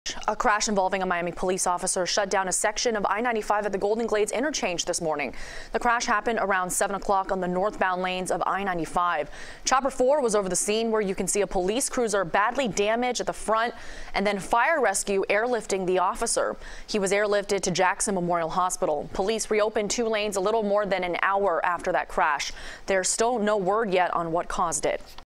Police Officer Involved Crash Shut Down Part Of Golden Glades Interchange
The injured Miami police officer was airlifted to Jackson Memorial Hospital.